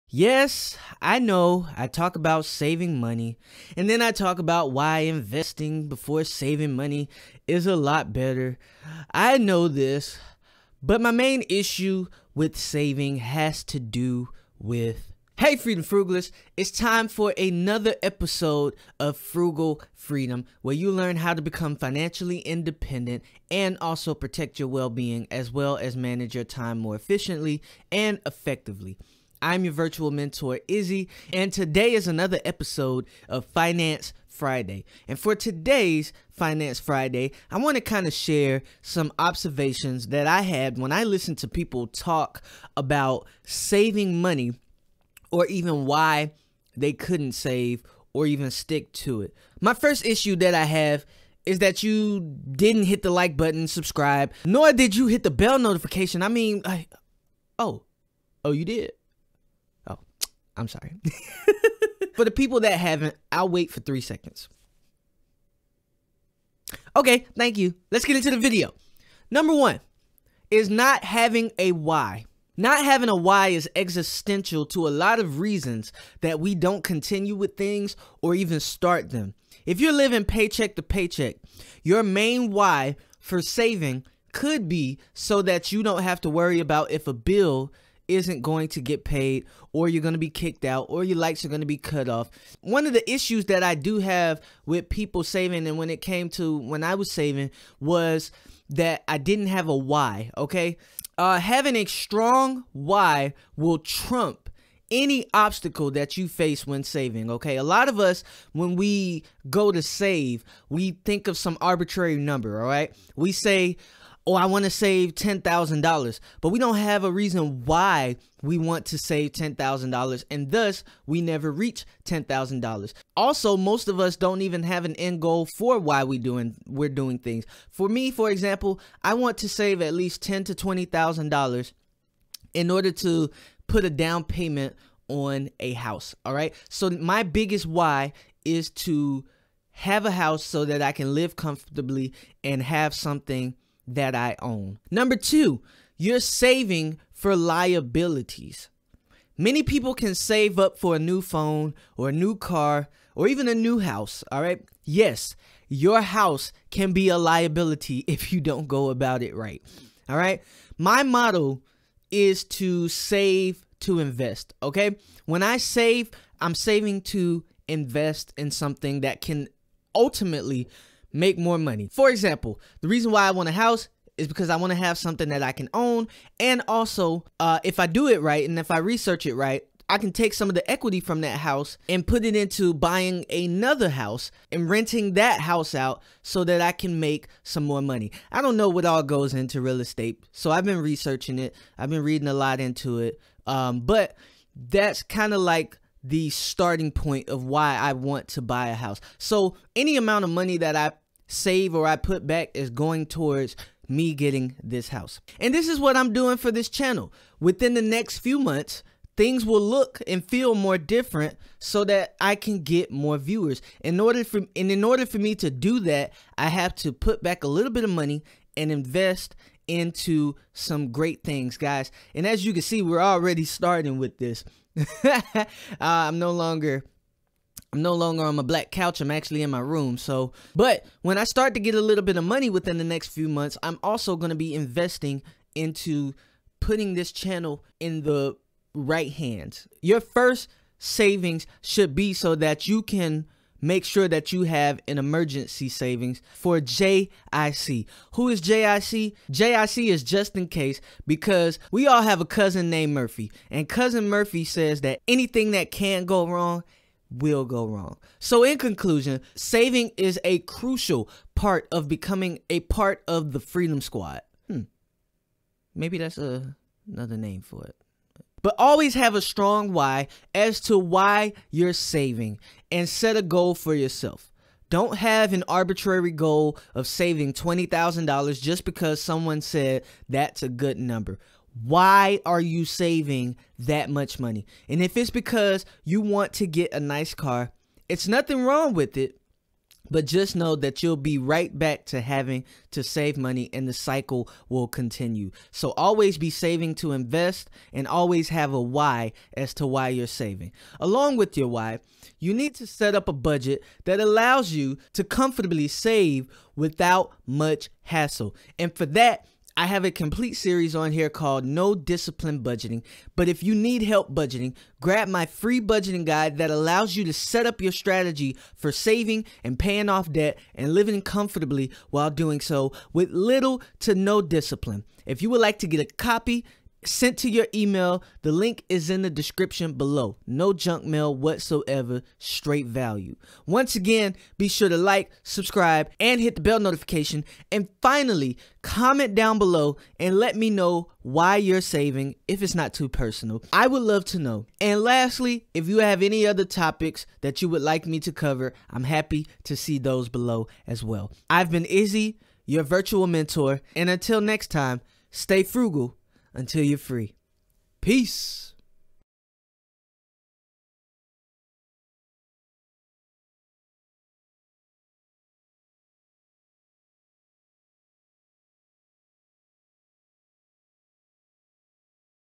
My ABSOLUTE Issue with Saving Money | Finance Friday
Of course I don't have a problem with saving money. It's just the way that we go about saving money...